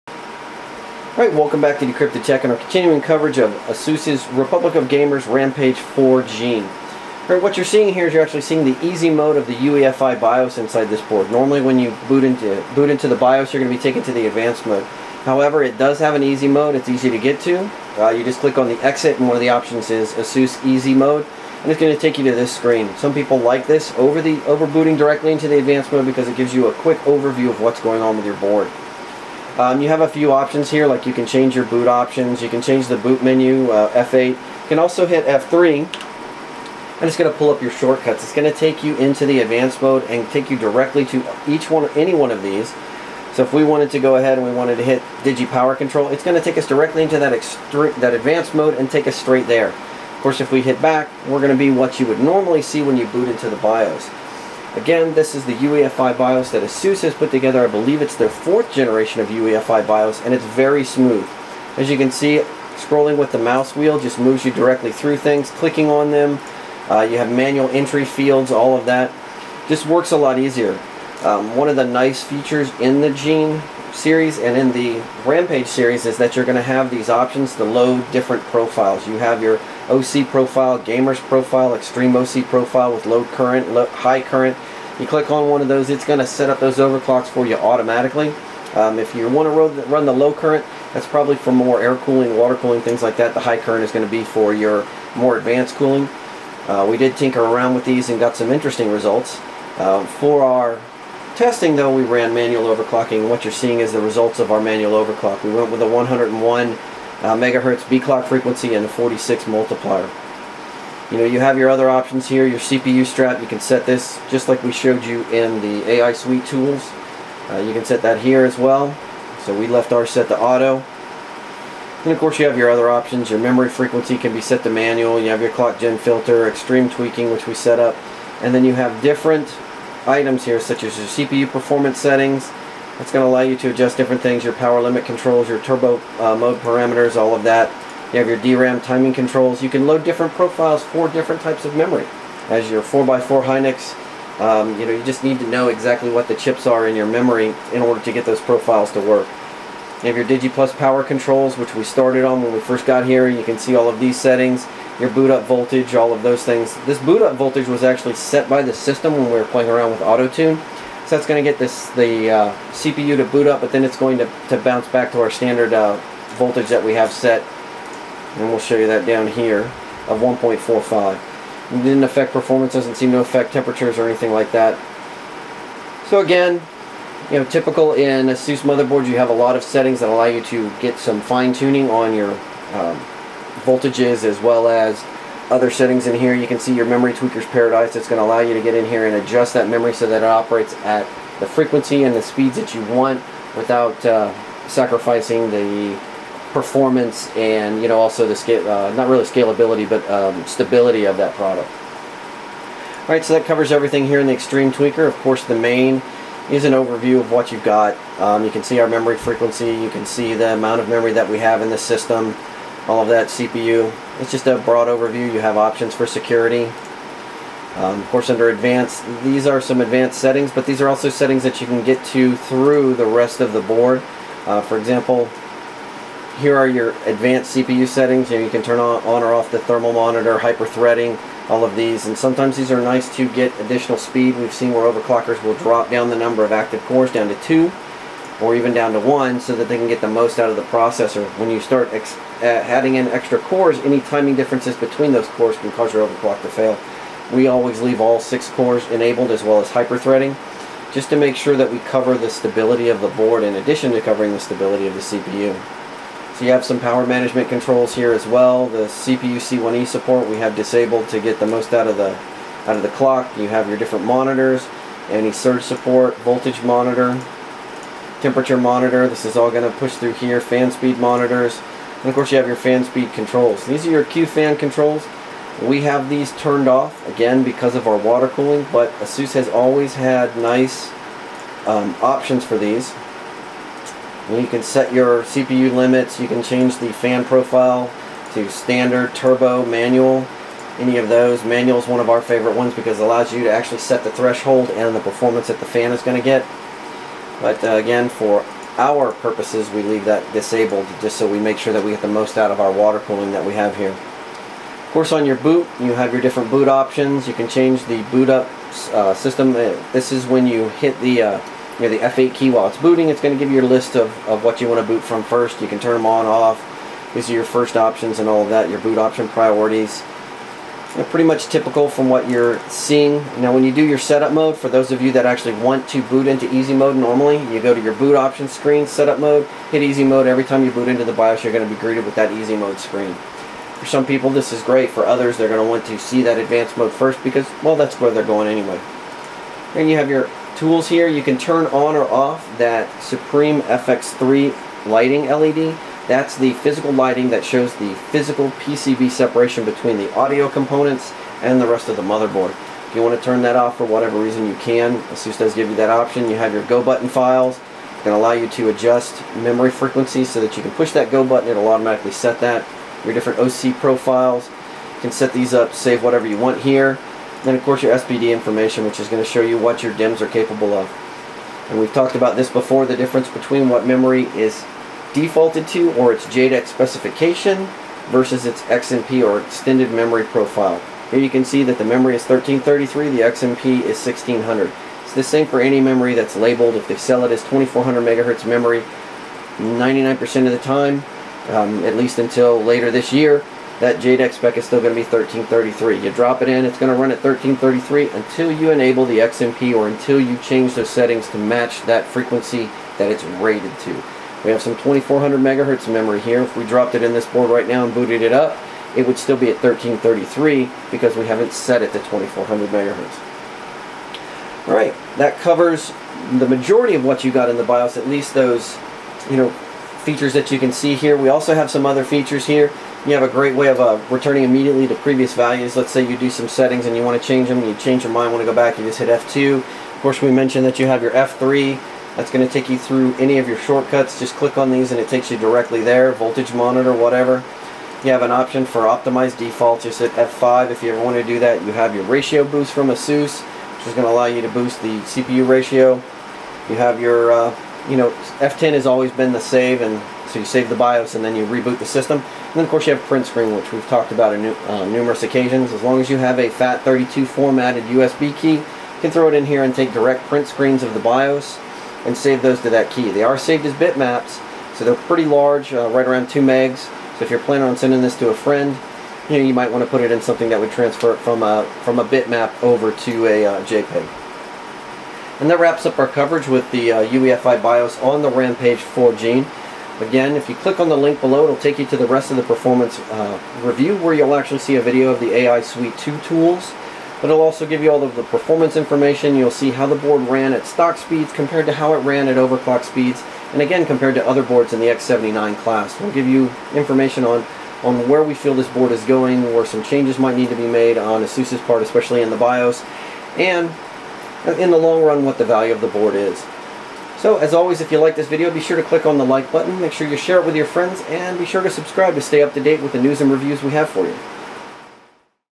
Rampage IV Gene UEFI BIOS
In our continuing coverage we take a look at the UEFI BIOS on the Asus Rampage IV Gene. Read the full performance Review at DecryptedTech